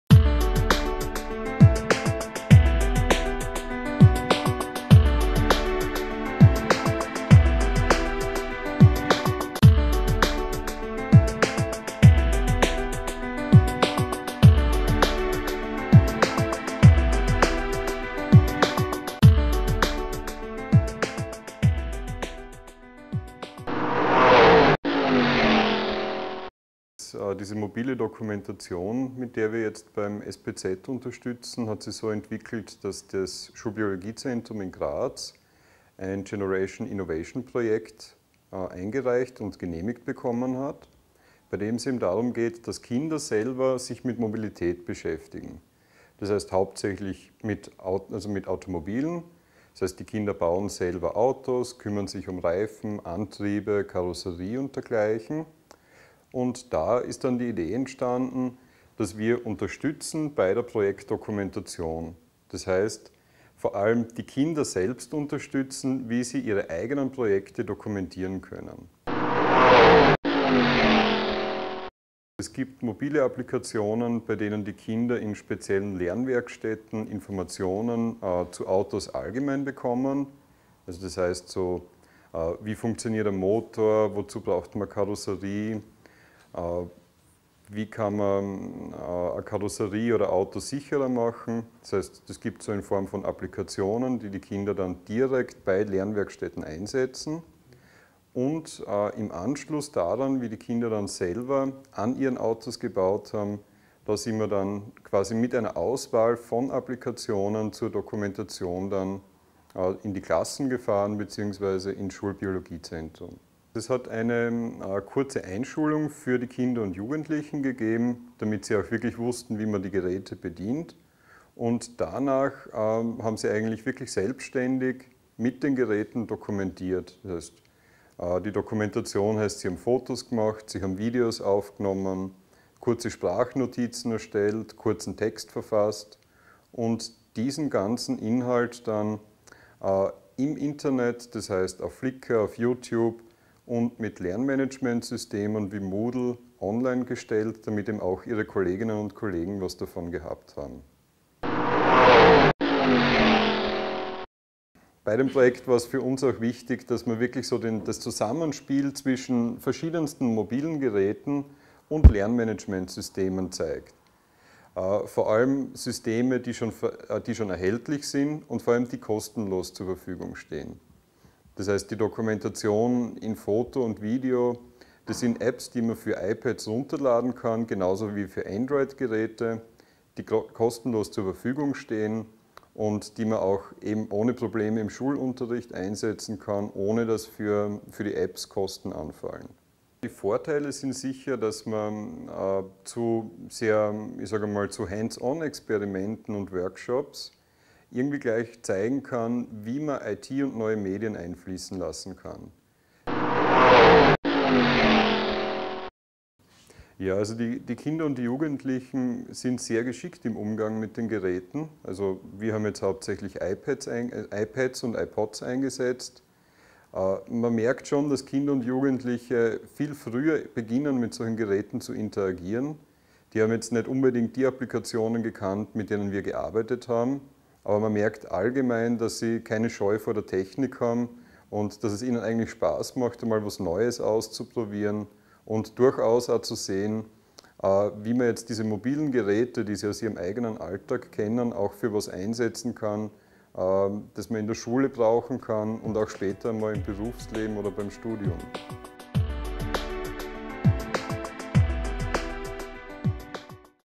mobiles Lernen und Dokumentation: Präsentationsvideo
Generation Innovation Projekt
Schulbiologiezentrum Graz

Video aus der Lehrveranstaltung DMT2
SBZ Dokmentation
Studierende: Gsöls Berghofer
Interview: Robert Strohmaier